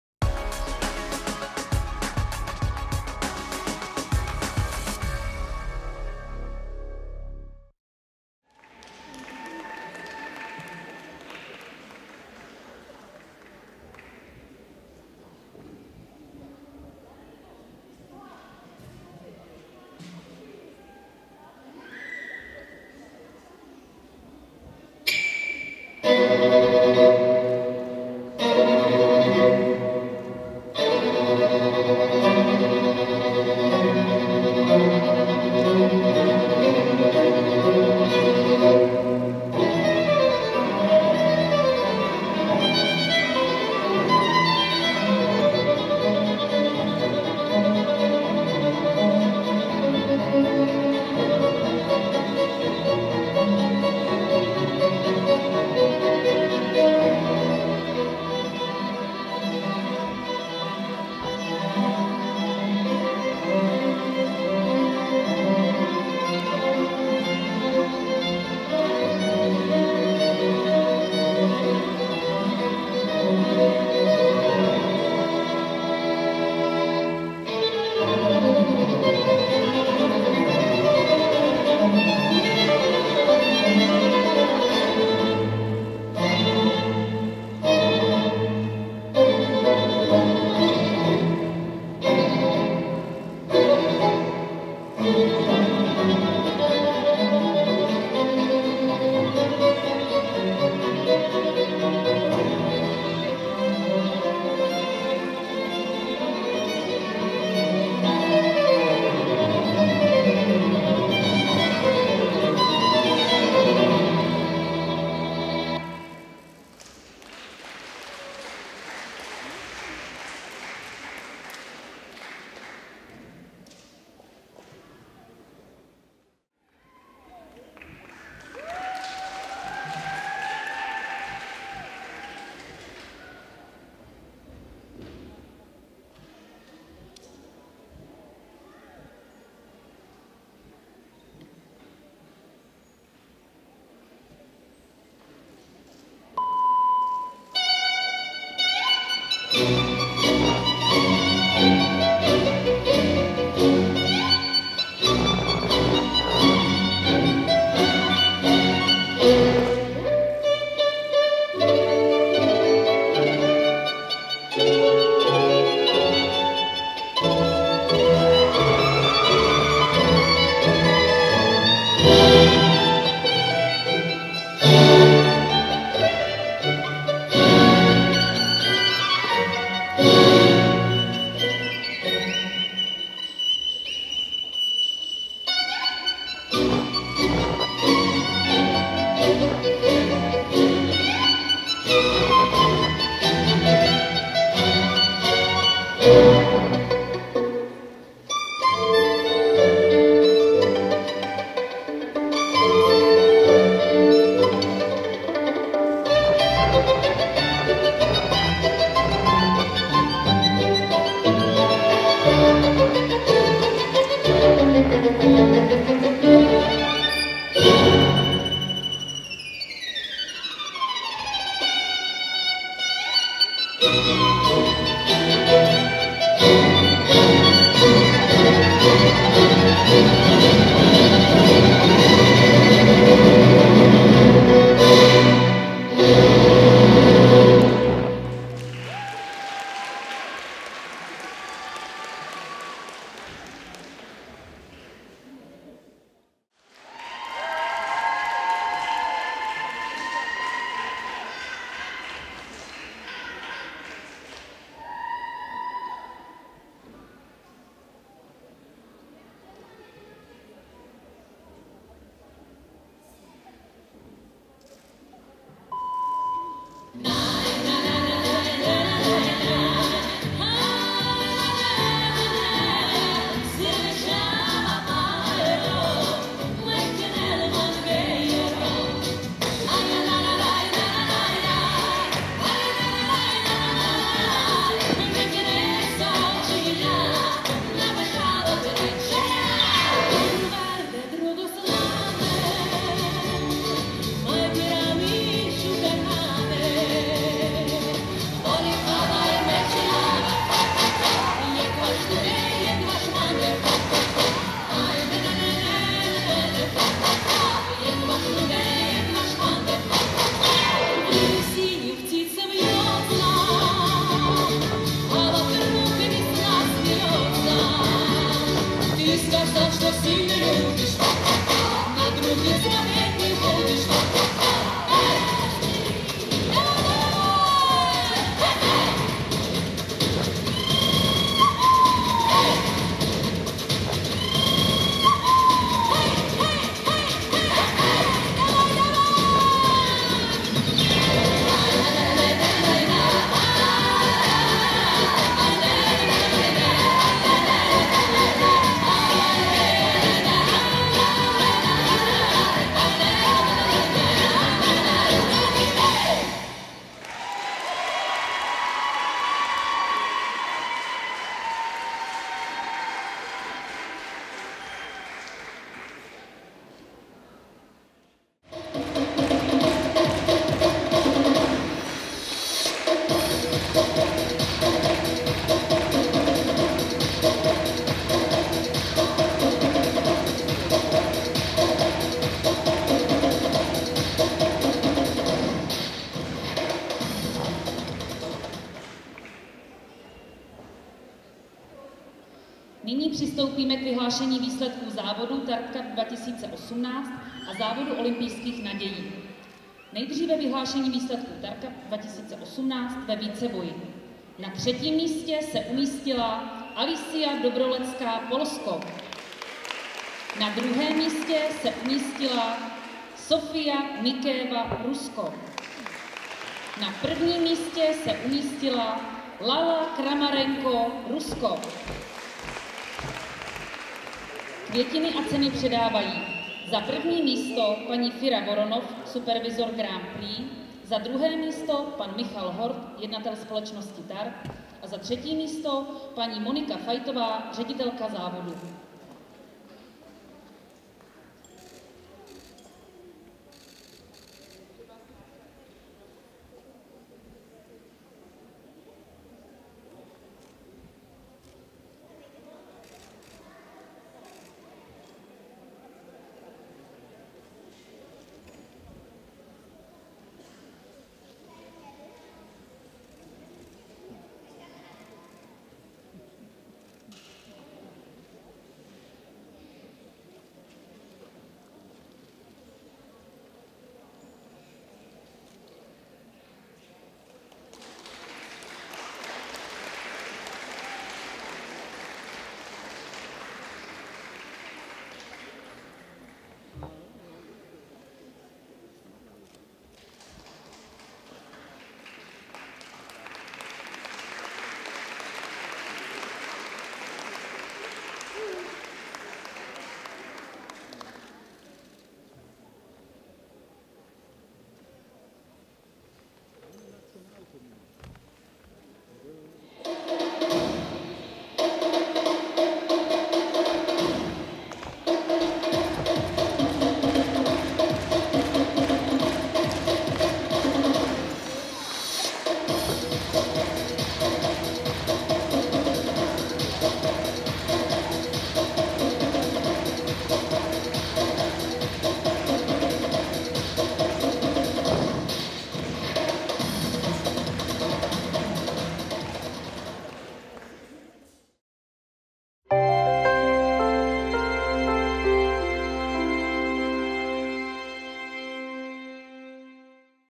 Tart Cup Brno 2018 - Junior Top 3 All Around
Tart Cup Brno 2018 - Junior Top 3: Lala Kramarenko (RUS), Sofia Nikeeva (RUS), Alicja Dobrolecka (POL)